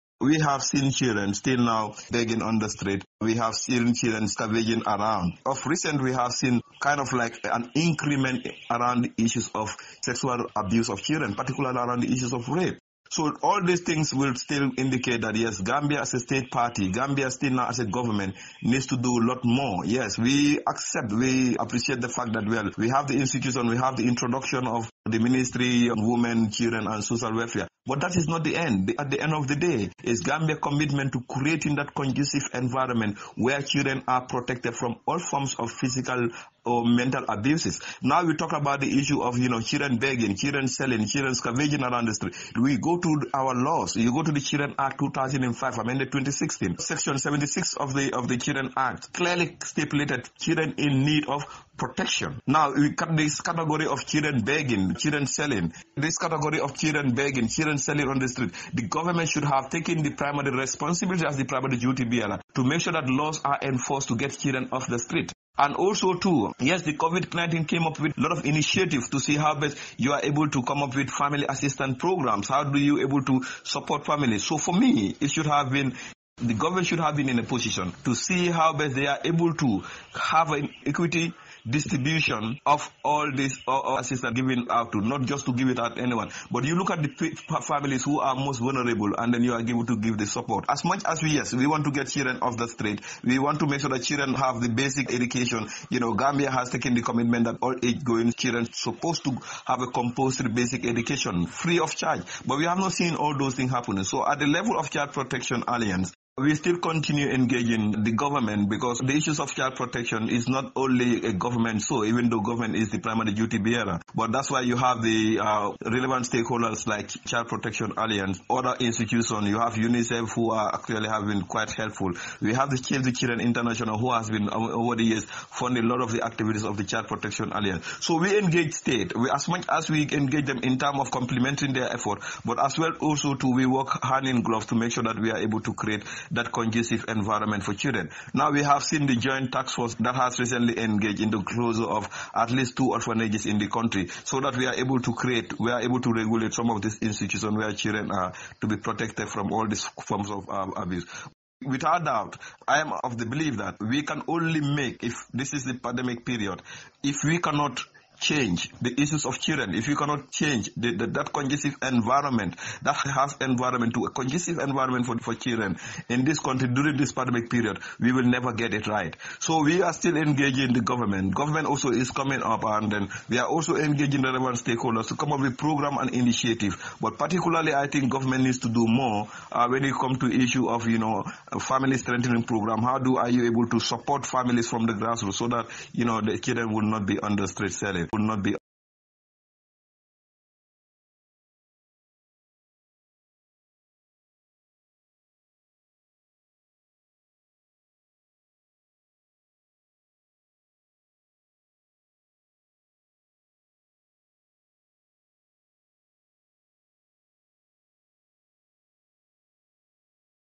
ENGLISHNEWS
Worldwide child welfare organizations warned that, covid-19 Lock down measures will increase cases of child maltreatment which includes sexual, physical and emotional abuse.
We now join Lamin Fatty, the National Coordinator of Child Protection Alliance (CPA), in a telephone interview with PTV, outlined some challenges children are facing during the pandemic and called on the government to intervene.